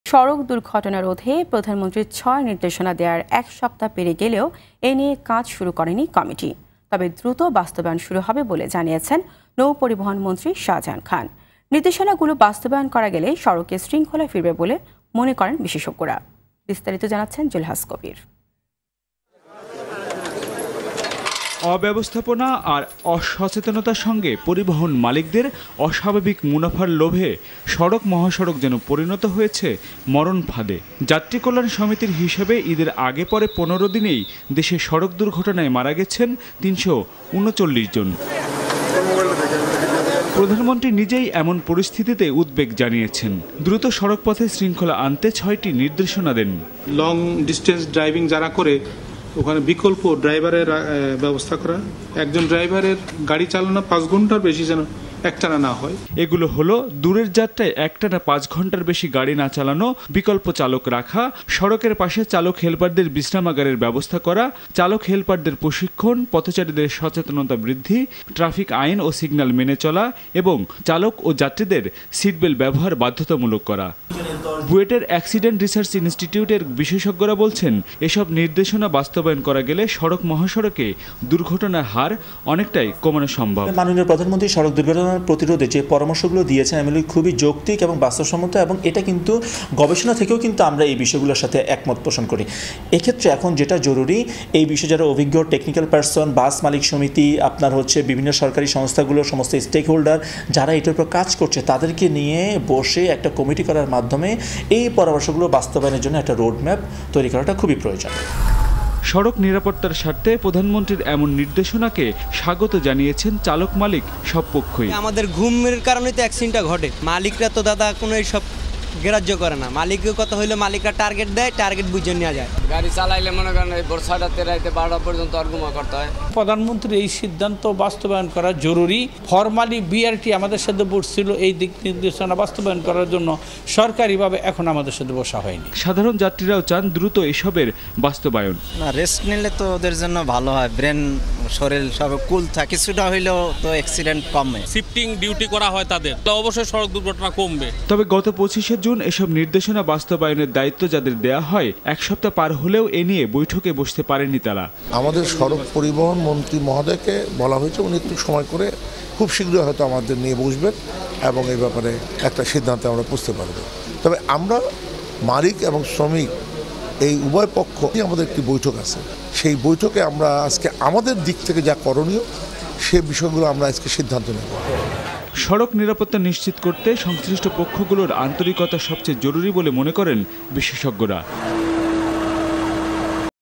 সড়ক দুর্ঘটনা রোধে প্রধানমন্ত্রী’র ছয় নির্দেশনার কাজ শুরু করেনি কমিটি
News : সড়ক দুর্ঘটনা রোধে প্রধানমন্ত্রী’র ছয় নির্দেশনার কাজ শুরু করেনি কমিটি

- Team RTV